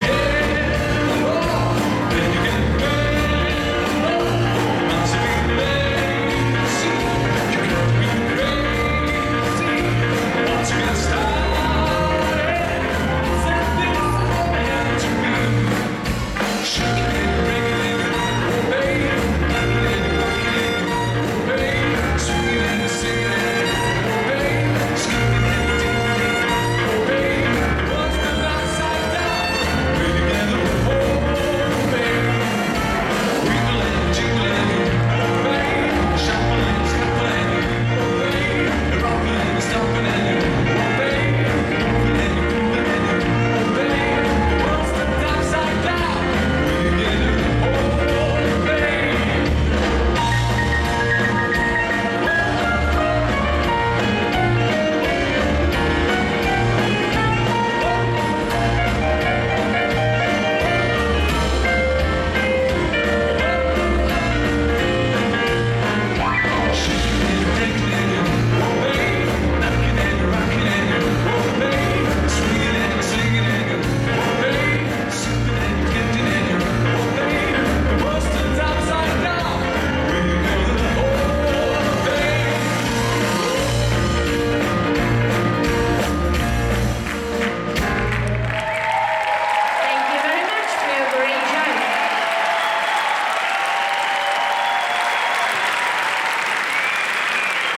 Grand Slam Latin 2011: Andrey Zaytsev - Anna Kuzminskaya - Jive Final
DanceMania.TV presents: WDSF Grand Slam Latin: 2011 -- Ostrava (Cz)